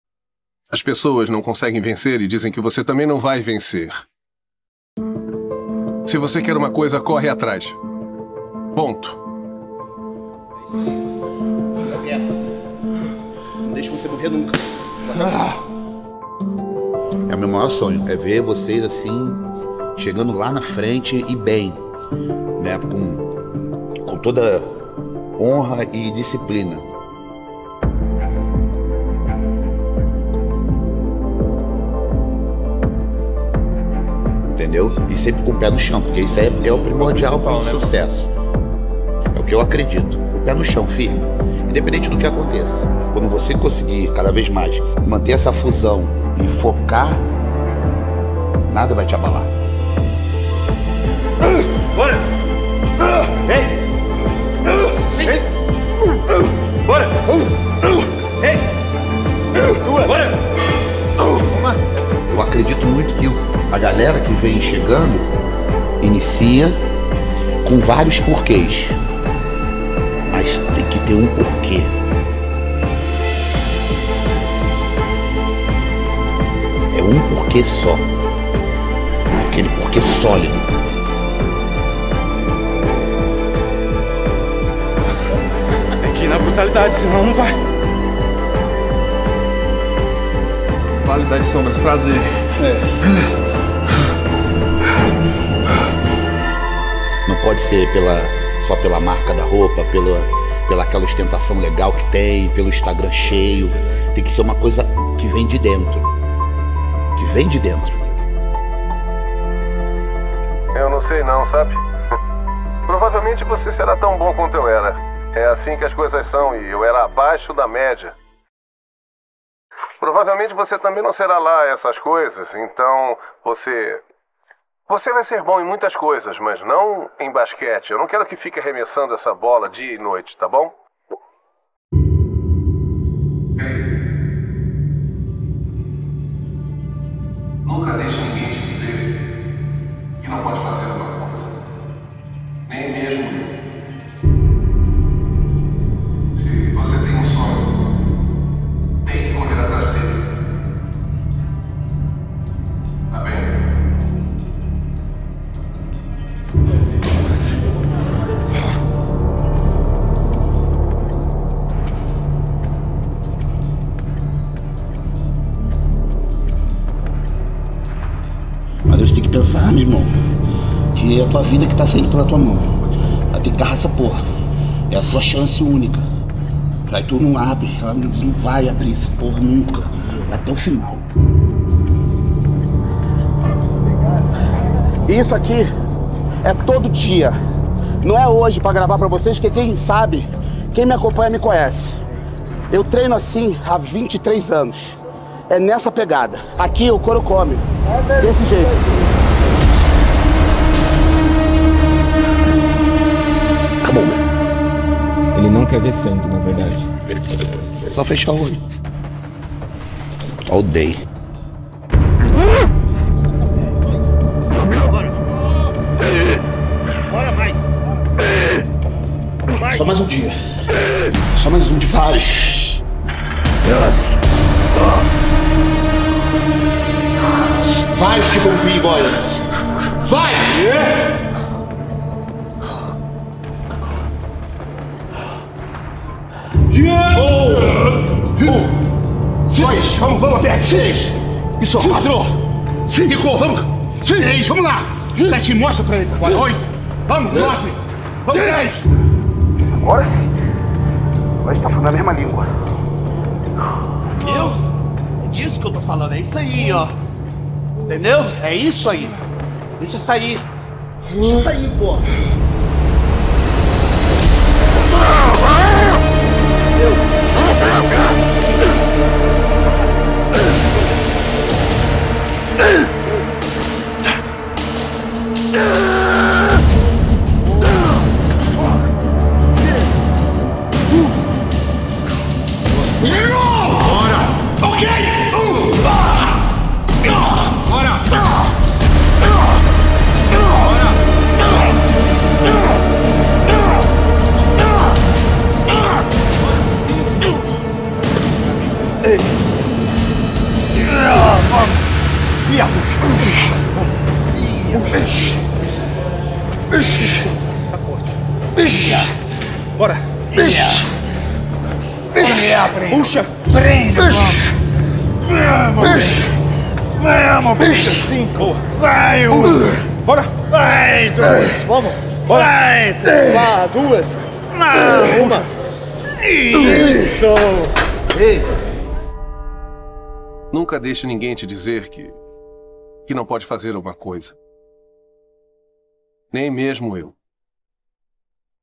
Jorlan Vieira - Motivação Bodybuilding | O porquê
Jorlan Vieira explica por que um atleta deve saber o seu porquê para atingir seus objetivos

Motivação sobre bodybuilding, fitness, treino, musculação, fisiculturismo e academia

Best motivational YouTube channel about bodybuilding, fitness, workout, gym, training

➡️QUER APARECER NOS VÍDEOS?

Capítulos
0:00 O porquê
2:16 All day